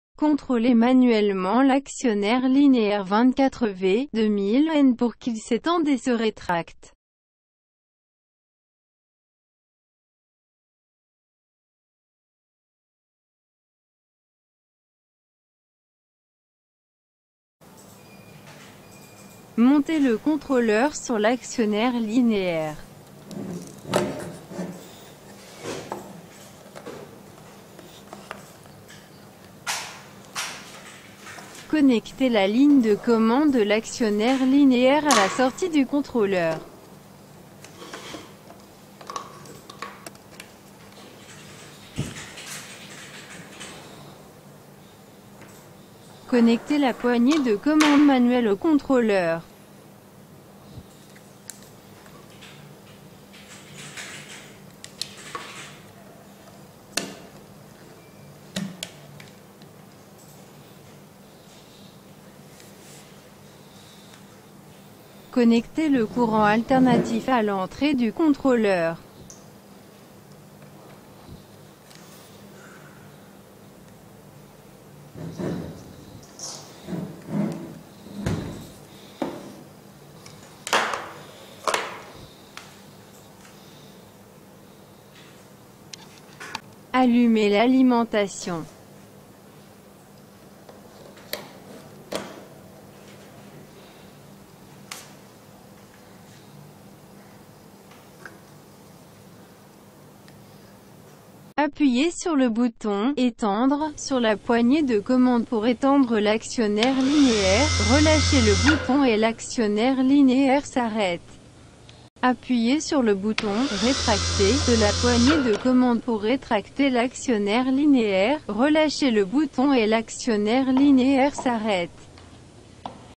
Comment utiliser interrupteur commande manuel avec poignée filaire pour contrôler vérin électrique
1 canal interrupteur commande manuel avec poignée filaire pour contrôler l'extension et la rétraction de l'actionneur linéaire électrique 2000N / 400 lbs.
Entrée AC 100V ~ 240V, sortie DC 24V / 3A.

Presser et maintenez le bouton «Extended»: L'actionneur linéaire s'étend vers l'extérieur; Relâcher le bouton, l'actionneur linéaire arrêt extension.
Presser et maintenez le bouton «Retracted»: L'actionneur linéaire se rétracte vers l'intérieur; Relâcher le bouton, l'actionneur linéaire arrêt rétraction.